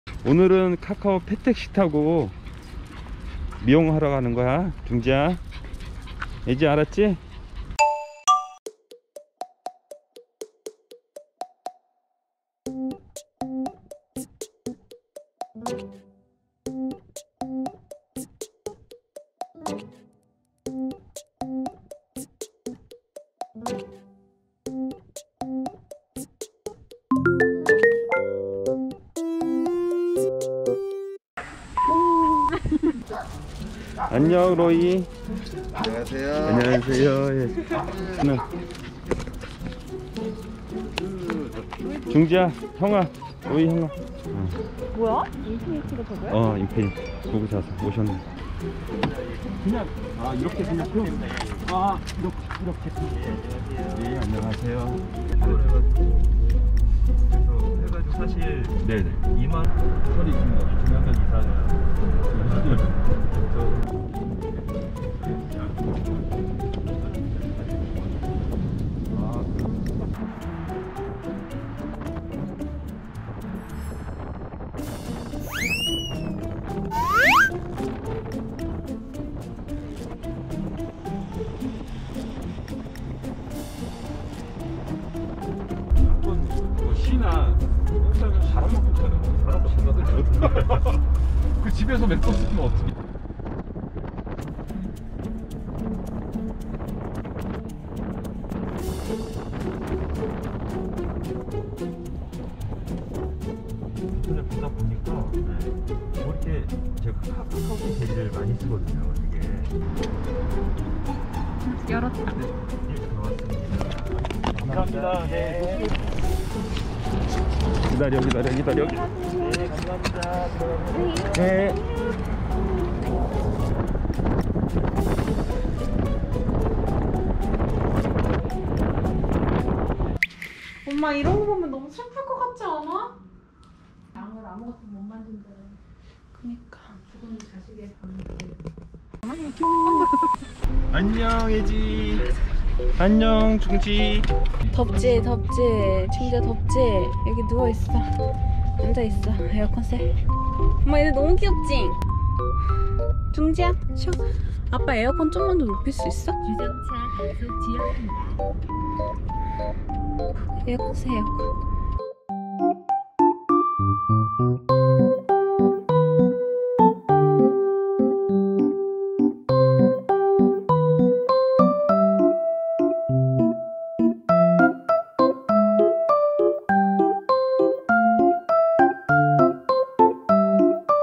카카오T 펫 첫 시승기! 우리 이제 맘 편히 택시타고 다녀요 ~ 반려인들을 위한 펫택시
처음으로 카카오 펫 택시를 이용해 봤어요 :)
만족도 상! 영상 함께 봐주세요♥